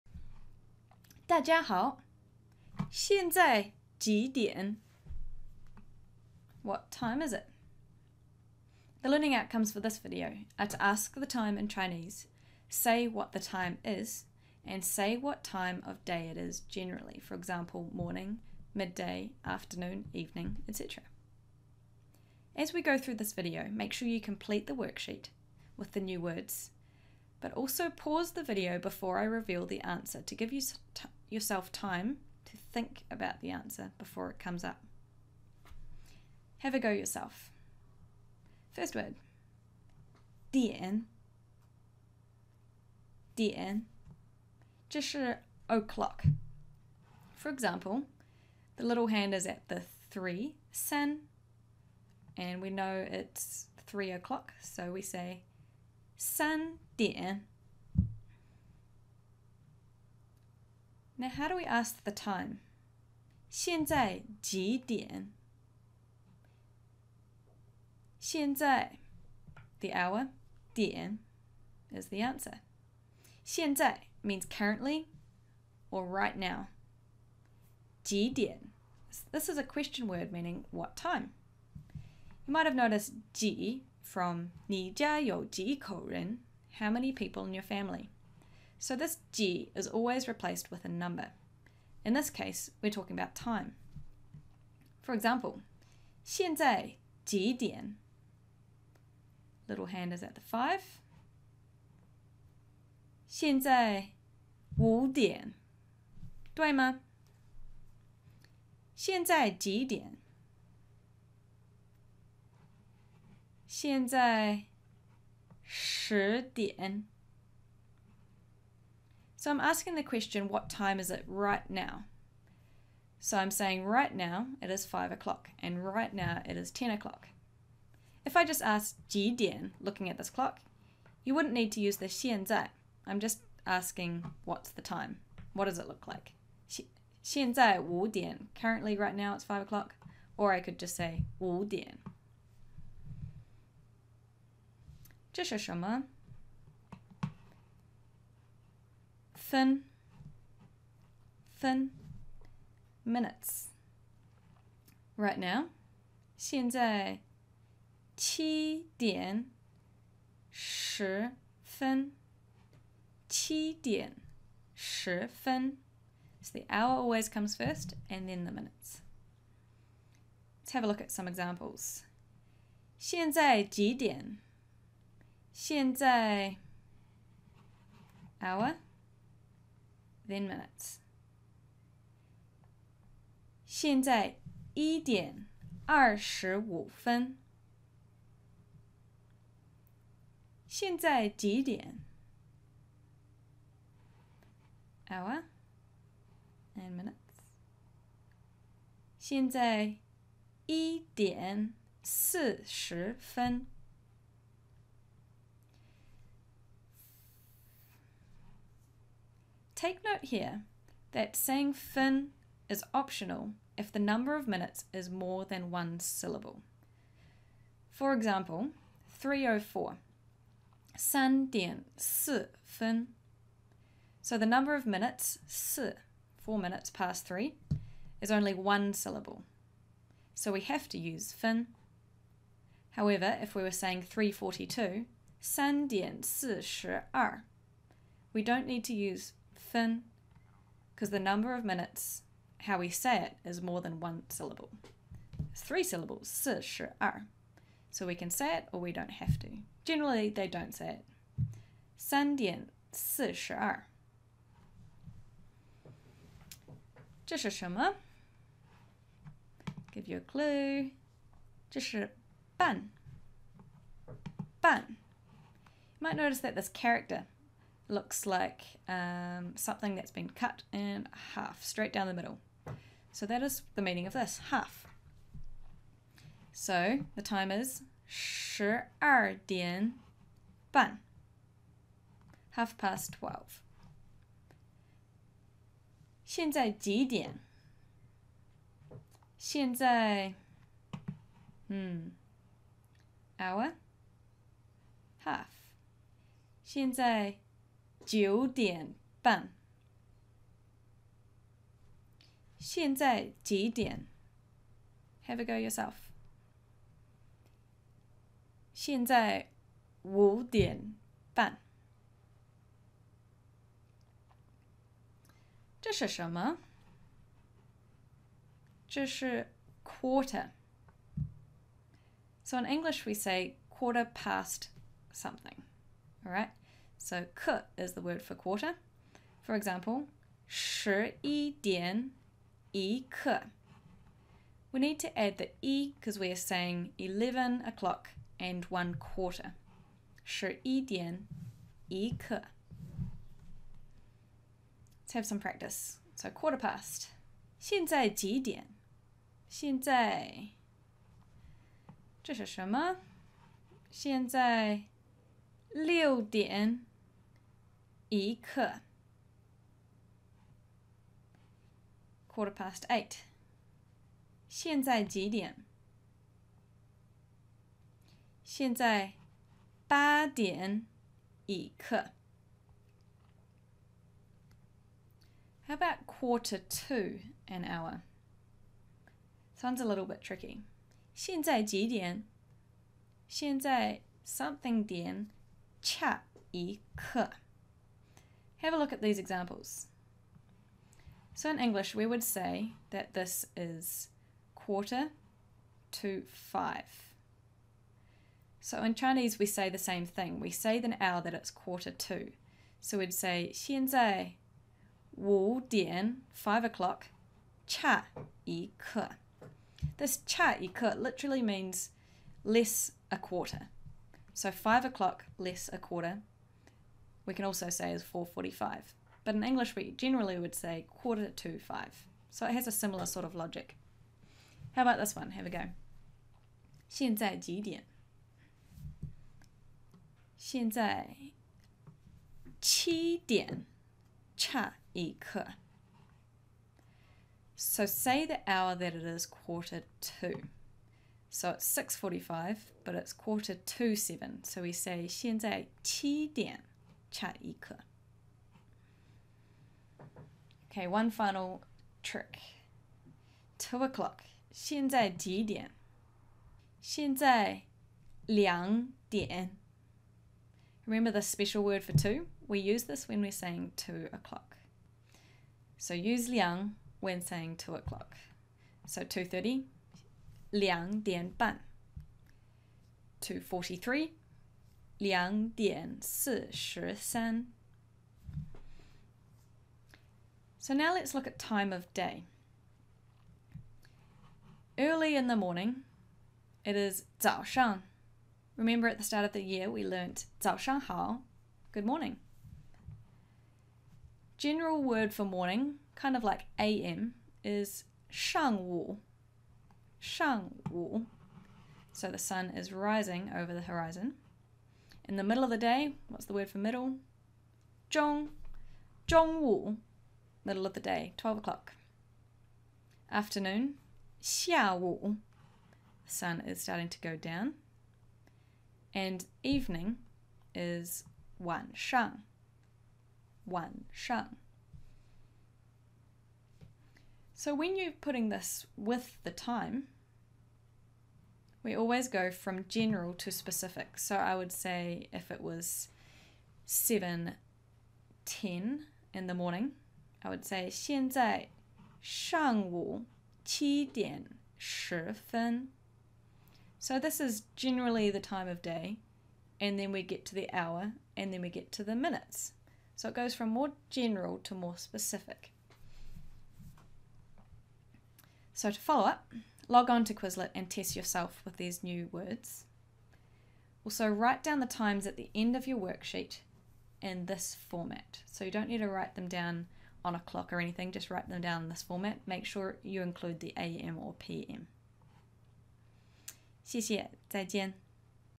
JinBu1 4.2 What time is it
A short video about asking the time in Chinese.